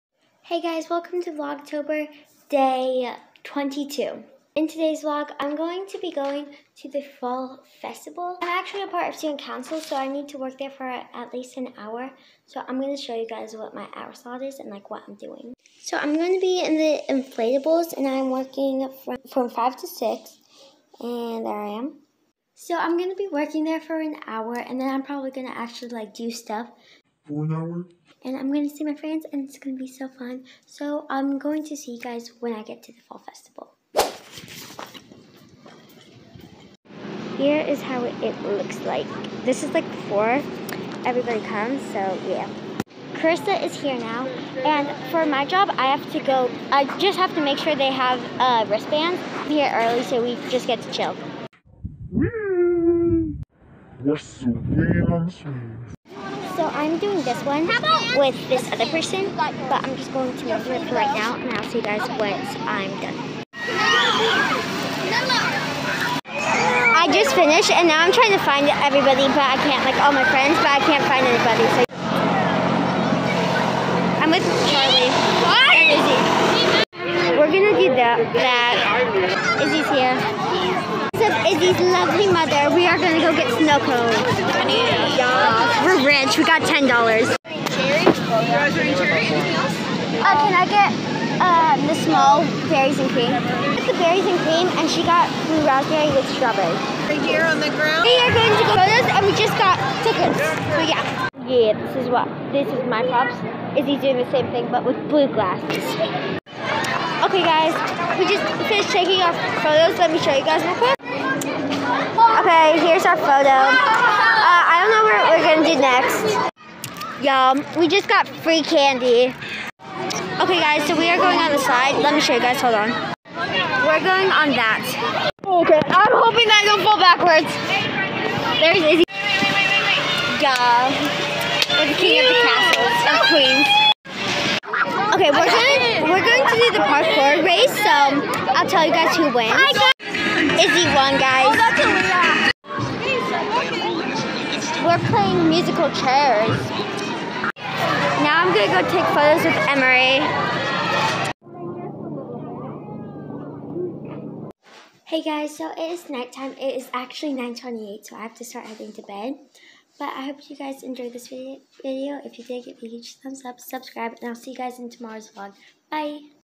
Fall Festival| VLOGTOBER DAY 22
🖤 Hey guys today was the fall festival!! Also sorry I didn’t film to much it was hard too.